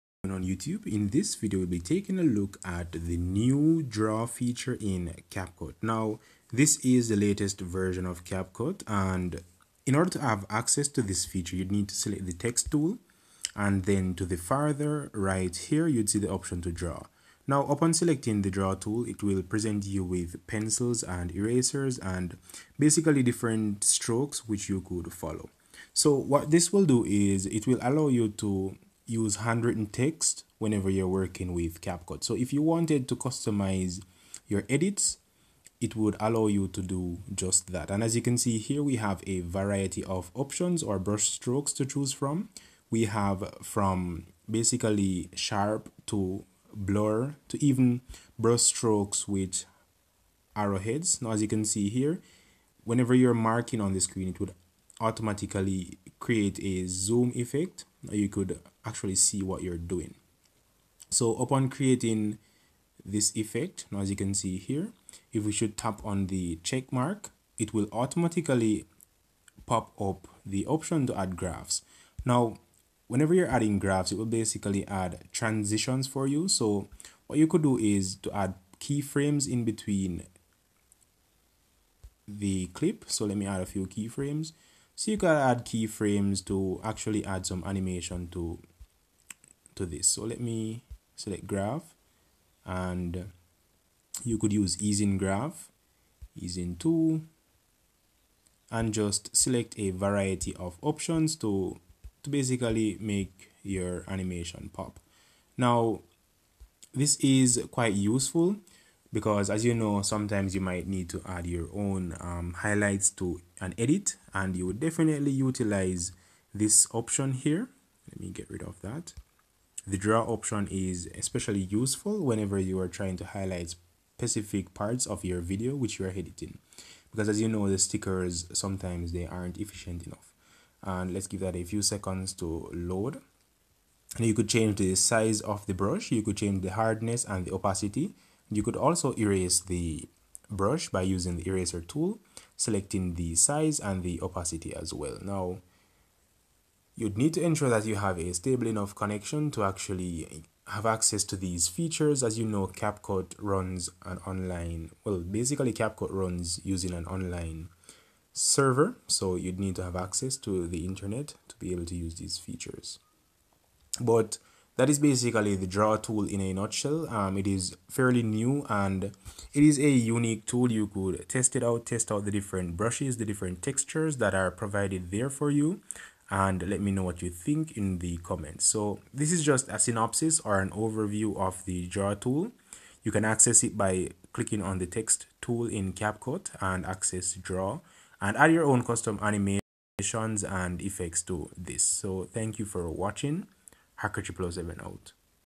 How to Use Draw Tool in CapCut | CapCut Video Editing Tutorial (Handwriting Tool CapCut)
In this video, we will explore the new CapCut draw tool and all the perks that are offered in the new update for CapCut.